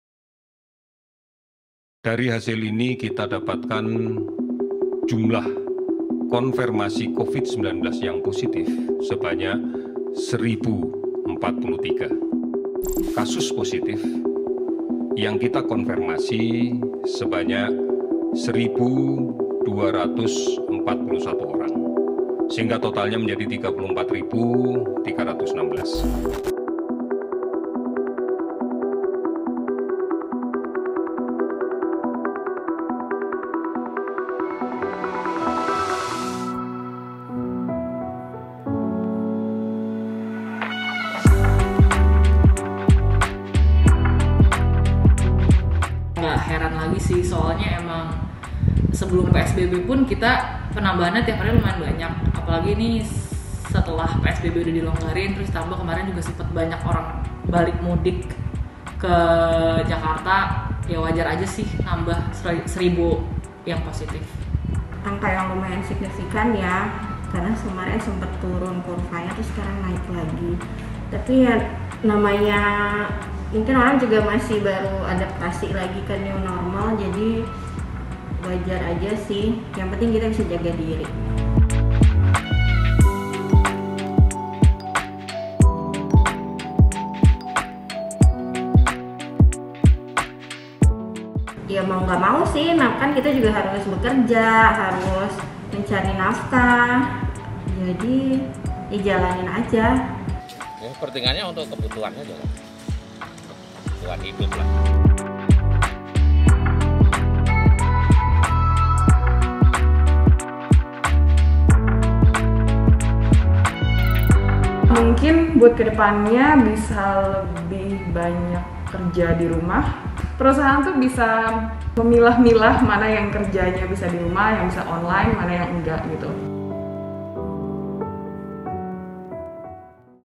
Dua Hari Kasus Positif Naik 1.000 Lebih, Apa Kata Pekerja Jakarta? | Narasi Newsroom
Harapan Para Pekerja Saat Kasus COVID-19 Masih Tinggi

Angka kasus positif COVID-19 tercatat naik lebih dari 1.000 dalam dua hari berturut-turut saat akan memulai penerapan normal baru.⁣
Ini membuktikan bahwa penyebaran virus corona masih terjadi di tengah masyarakat. Namun para pekerja tak punya pilihan, mereka tetap bekerja di kantor demi memenuhi kebutuhan ekonomi.⁣
Mereka pun berharap perusahaan bisa merumuskan mekanisme kerja untuk menjamin keselamatan karyawannya.⁣

(Narasi)